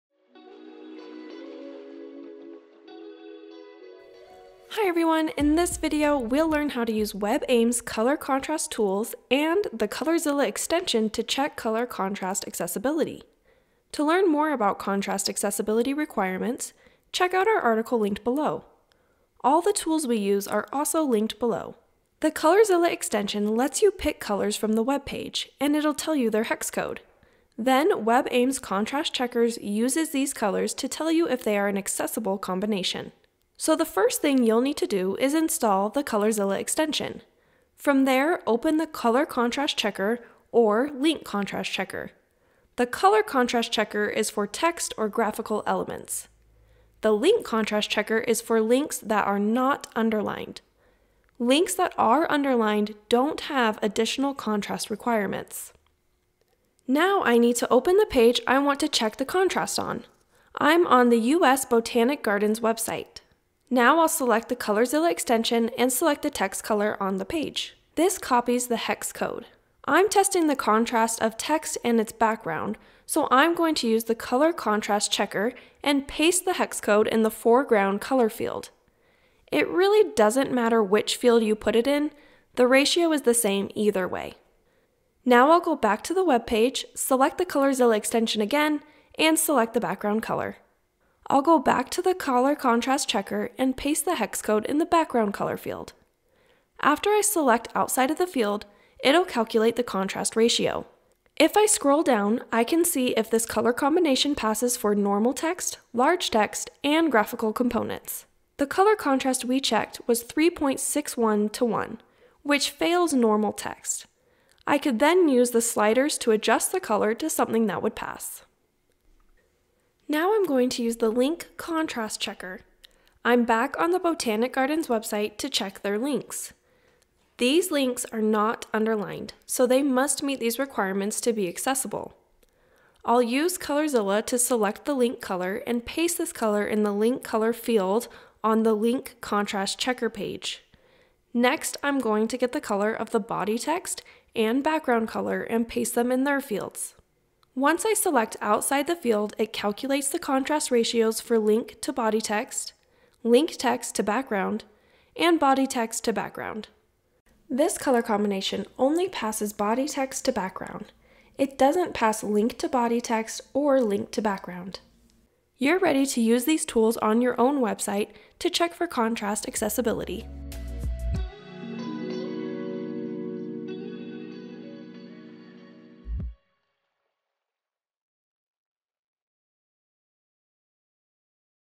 Fix contrast issues with WebAIM Contrast Checkers and ColorZilla
Use WebAIM's contrast checker and link contrast checker with ColorZilla to find and fix contrast issues. Learn more about contrast issues in our article or monthly focus.

Chapters:
0:00 Introduction
0:22 ColorZilla and Contrast Checker overview
0:56 Use ColorZilla and WebAIM's Color Contrast Checker
2:00 Use ColorZilla and WebAIM's Link Contrast Checker